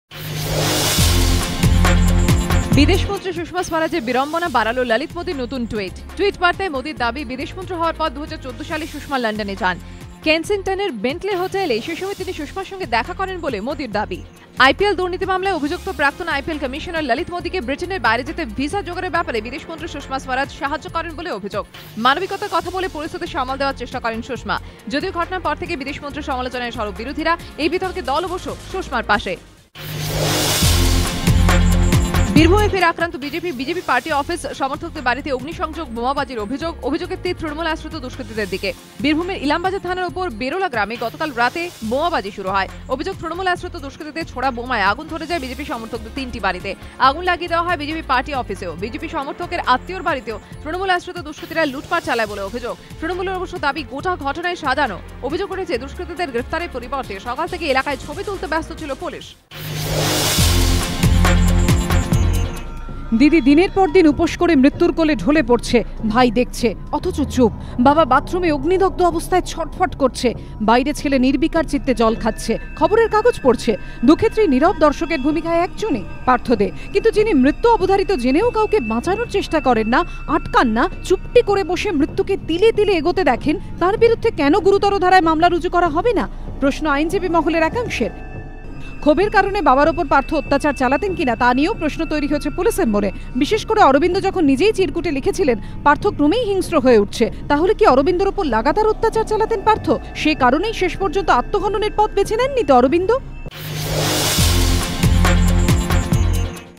3 মিনিটে এই মুহুর্তের বড় খবর
Lalit Modi  tweets he met Sushma Swaraj in London hotel last year.